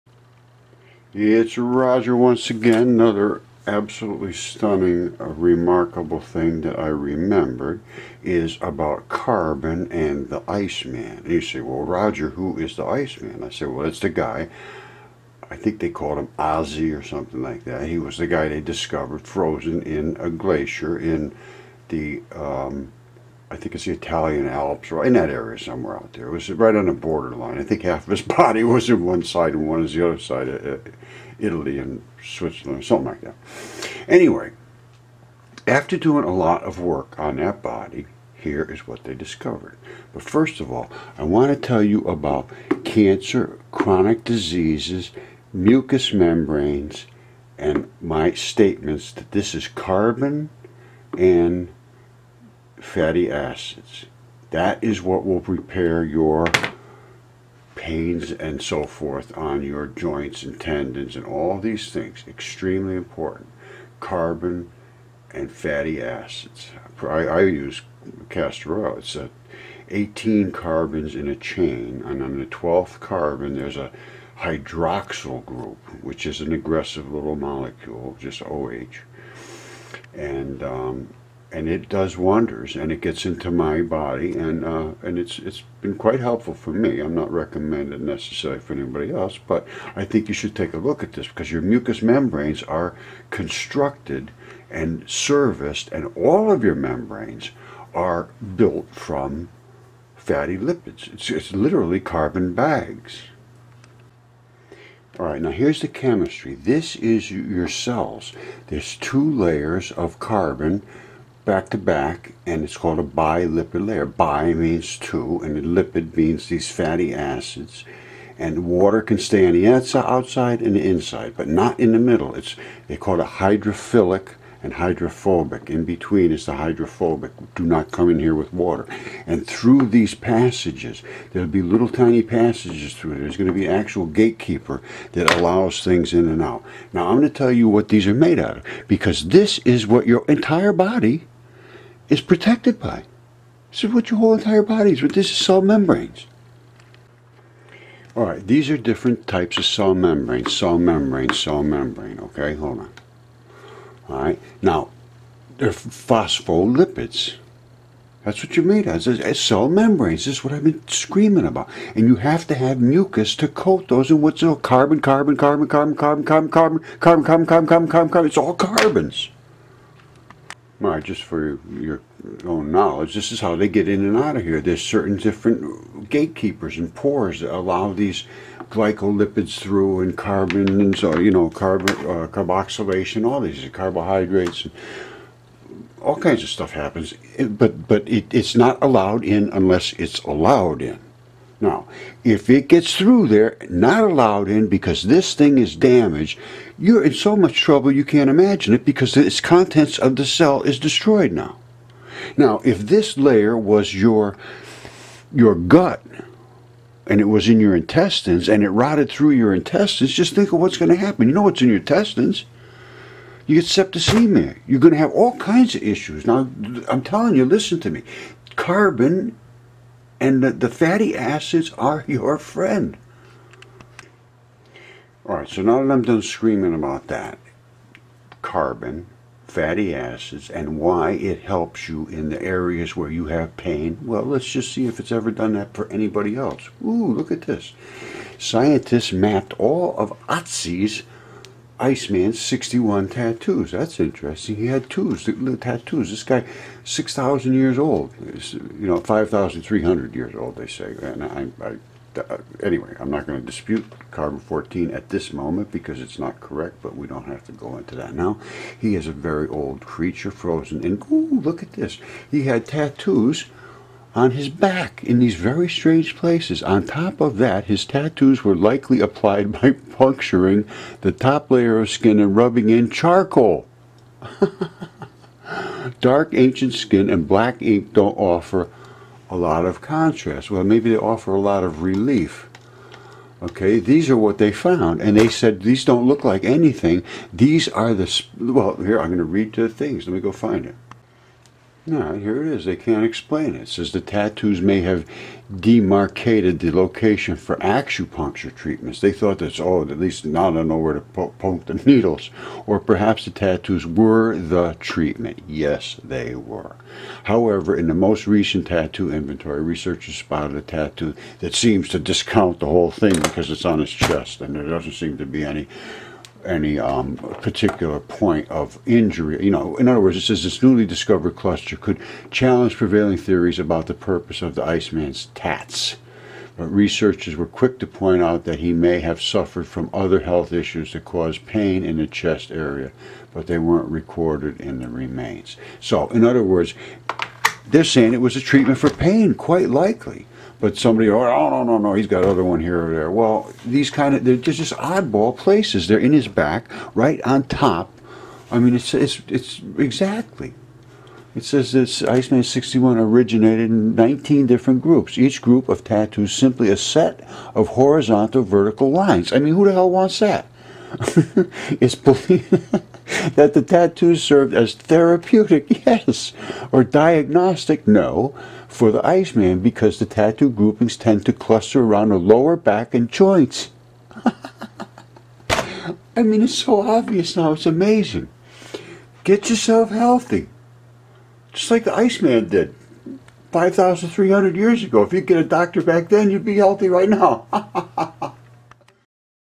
Otzi the Iceman used Carbon for Pain
My research into the membranes that are your immune system led me to carbon. All membranes have carbon and host bacterial colonies that cooperate to produce products and mucus.
In my other videos I show the inter action and the consequences of antibiotics and gastro intestinal and other issues that result in cancer. The result is that the mucus membrane that separates acids and feces and such breaks down from lack of fatty acids including carbon. It appears Otzi knew this.